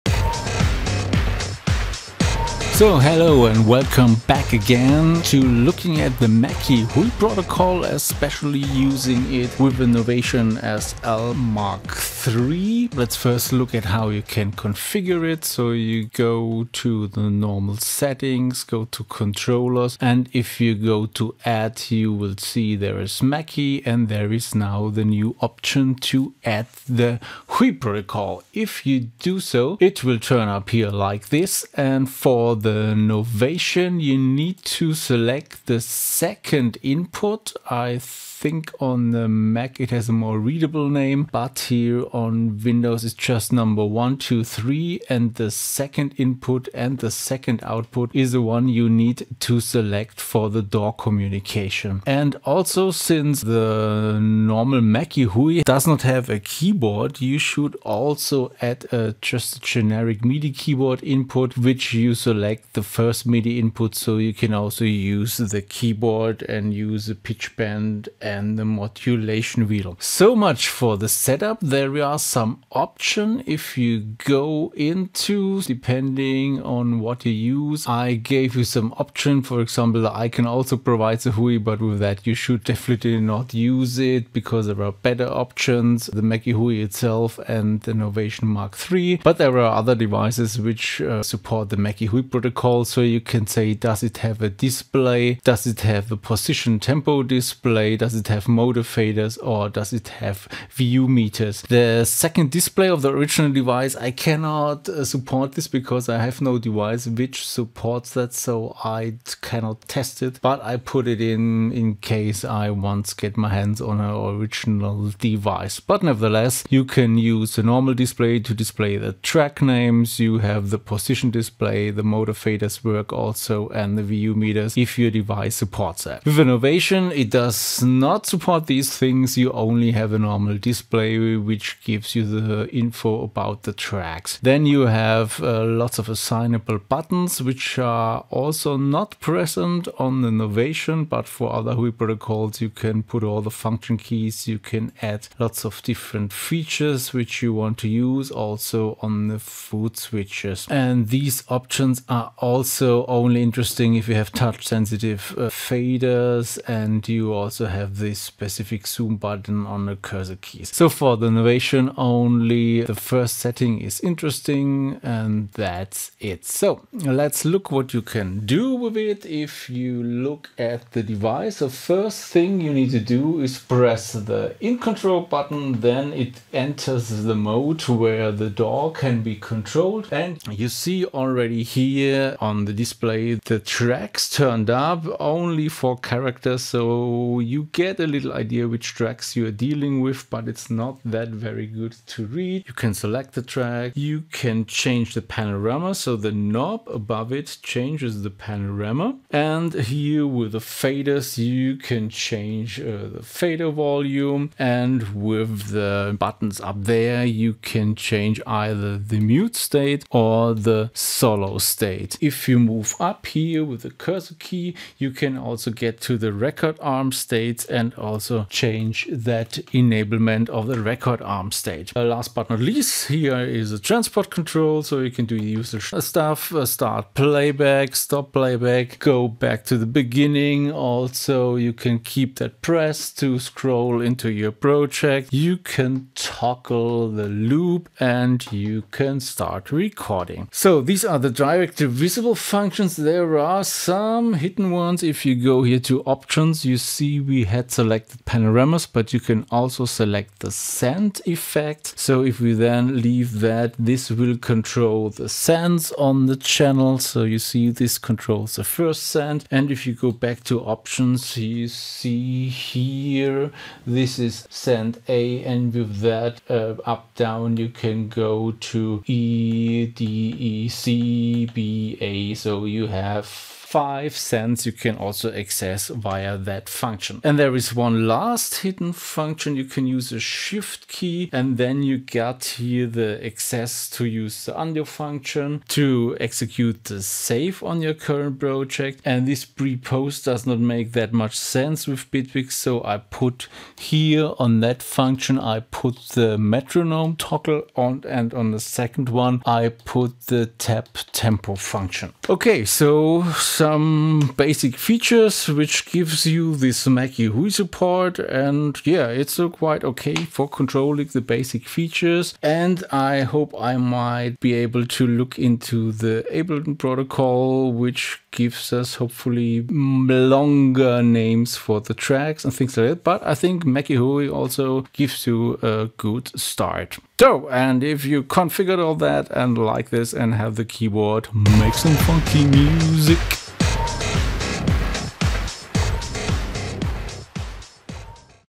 Mackie HUI Protocol with Novation SL MkIII
Bitwig Studio tutorial which shows the Mackie HUI protocol provided by DrivenByMoss 6.0 with the Novation SL MkIII.